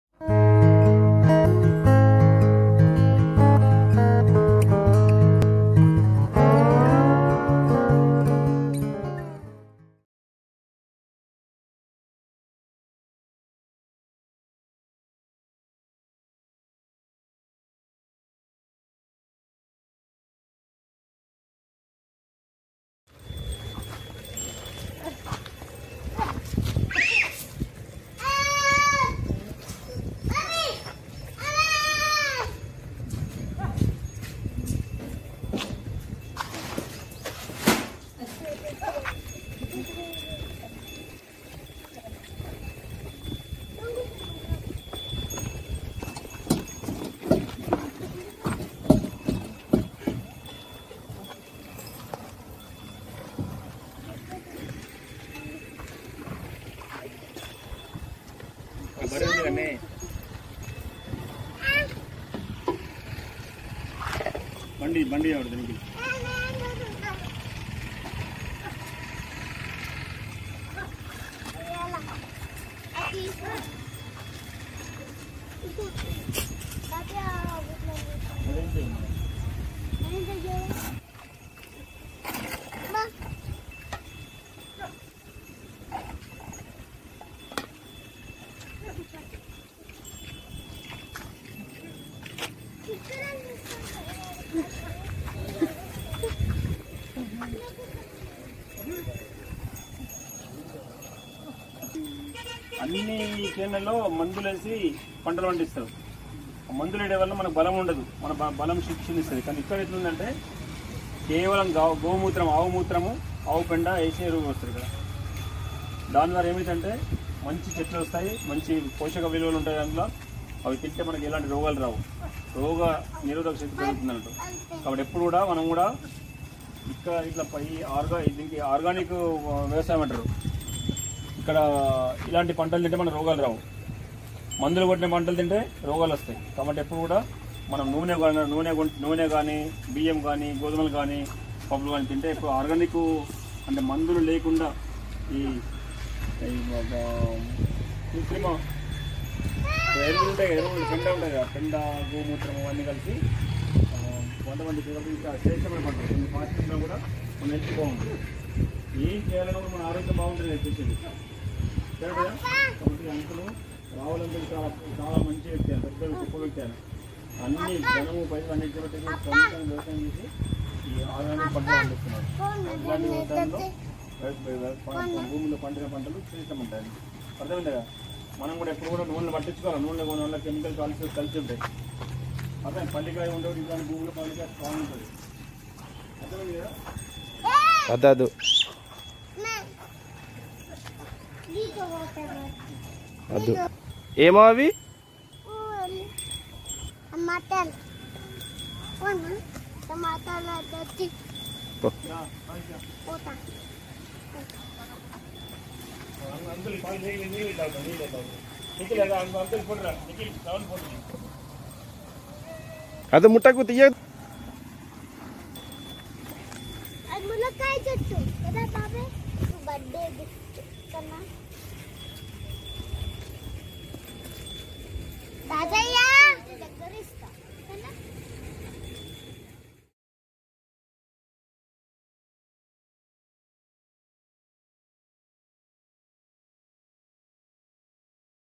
organic farm tour part 2 | Vedansh Reddy Vlogs
Welcome to Hello Vedansh Youtube Channel,  visited organic farm at bhainsa and spent a good time there.

Thank you Purastu Rahul Bro for making convenient .Purastu Rahu Bro is a Classical Musical Teacher and he is very much interested in doing organic farm, For organic Products Contact us @ .